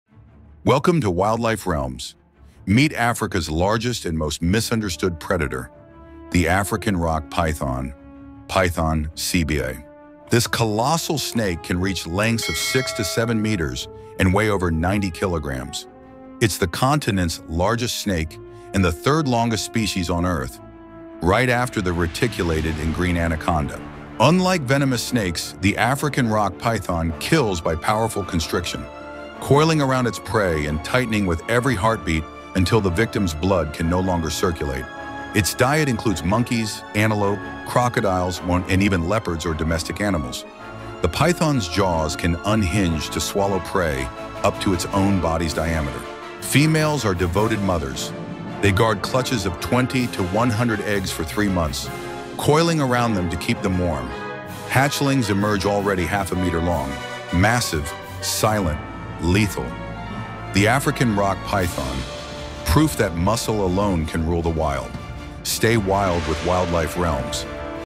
The African Rock Python: The Deadly Power of Africa’s Largest Snake🐍💀rockpython
Scientific Name: Python sebae
Average Length: 4.5–5.5 m (typical adults)IUCNMaximum Recorded Length7.5 m (verified record in Sierra Leone)Smithsonian
Weight: 45–90 kg; exceptional less110 kg
NatGeoBody Diameter: Up to 30 cm
Herpetological Review Hunting Method Ambush constriction; uses infrared-sensitive pits to detect warm-blooded prey Animal Diversity WebDietMammals, birds, reptiles; occasionally large prey like antelope, crocodiles, or leopards
SensoryHeat-sensing pits on labial scales detect 0.003 °C changesPNAS
Reproduction Oviparous; 20–100 eggs; incubation ~85 days; female thermoregulates clutch via shivering Smithsonian
Lifespan20–30 years (wild), up to 40 (captivity)IUCN
Habitat: Sub-Saharan Africa — savannas, forests, near rivers and rocky escarpmentsWWF
Behavior Nocturnal, semi-aquatic; strong swimmer; often basks in morning sun
NatGeoConservation Status Least Concern (IUCN 2024) globally, but declining in West Africa due to hunting for skin and bushmeat IUCN Red List
Muscular Power Can exert over 6000 newtons of constriction force — enough to stop a human heart in seconds
🌍 Scientific Highlights:
• The African Rock Python has two subspecies: Python sebae sebae (mainland) and Python sebae natalensis (southern Africa).
• Its genome shows genes linked to extreme muscular endurance and bone density, allowing sustained constriction.
• Infrared-sensing labial pits enable detection of prey in total darkness.
• It can hold its breath for over 30 minutes underwater while ambushing near rivers.
• Juveniles mimic venomous snakes like cobras to deter predators — a behavior known as Batesian mimicry.
🎧 Editing & Sound Suggestions:
• Start: Distant thunder and heartbeat rhythm synced with camera zoom on snake’s eyes.
• Middle: Slow-motion wrap around prey with deep bass pulse.
• End: Fade-out with hiss sound + jungle echo, then channel logo Wildlife Realms.

The African Rock Python is Africa’s largest snake — a muscular giant that kills its prey with unstoppable force. 🐍💪
Reaching up to 6 meters (20 feet) long, it can take down antelopes, monkeys, and even young crocodiles. 🐊😱
It strikes in silence, coils tightly, and waits… until the jungle goes still. 🌿
🔥 In this short, witness the power and mystery of the African Rock Python — the fearless serpent of Africa’s wild heart!
🌿 Explore more amazing wildlife every day on Wildlife Realms.